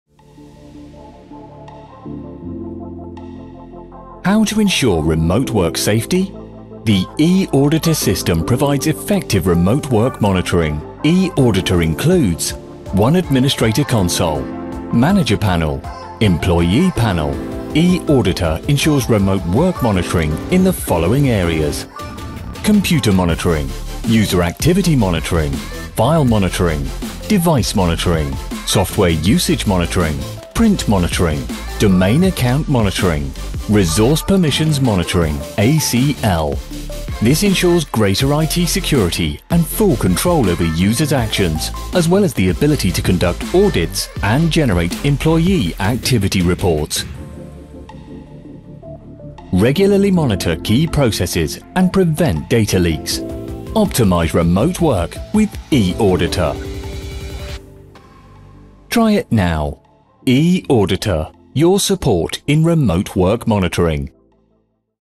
eAuditor - IT Monitoring | Remote Work Security
The eAuditor system provides comprehensive support for remote work monitoring, utilizing the following functionalities:

🖥️ computer monitoring,
👥 user activity monitoring,
📂 file monitoring,
🖱️ device monitoring,
🛠️ software usage monitoring,
🖨️ print monitoring,
🔐 domain account monitoring,
🚀 ACL resource permission monitoring.

Work efficiently and securely with the eAuditor system!